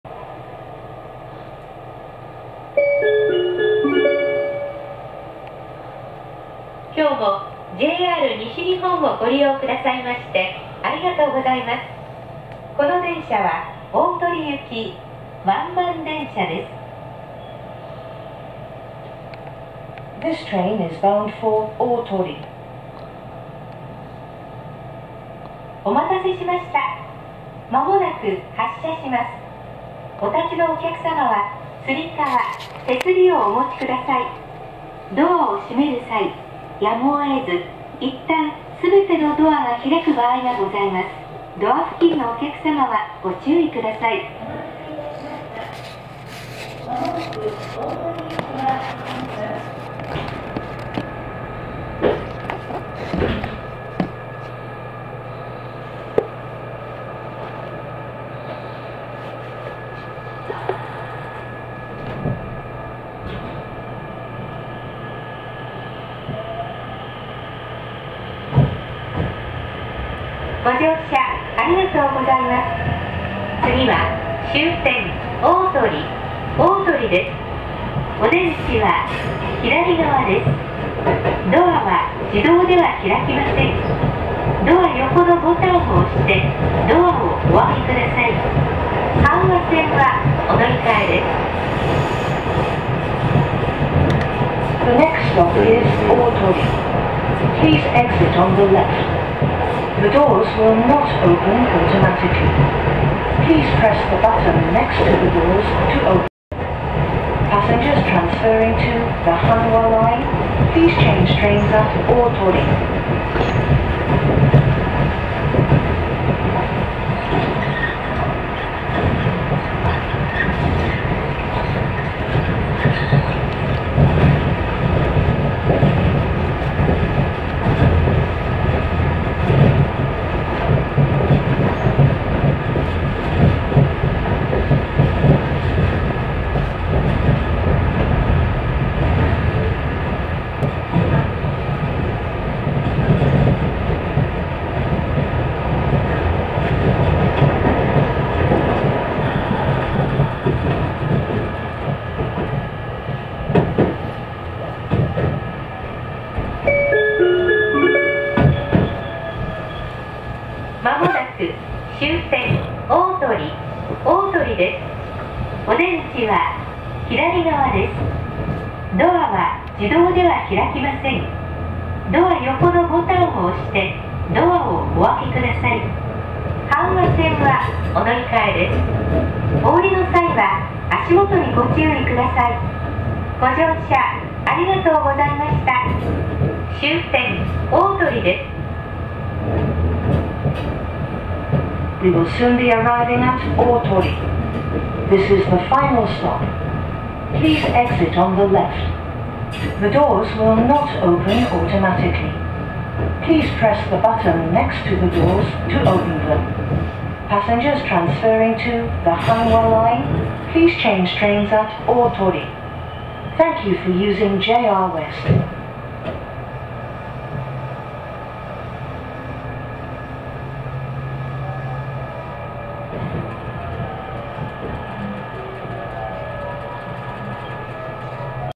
ST131 ep10'1 山田の車窓から：JR羽衣線・東羽衣駅→鳳駅【JR西日本ターミナル駅完全制覇の旅～阪和編～】
〒592-0003 大阪府高石市東羽衣１丁目１８
JR羽衣線　東羽衣駅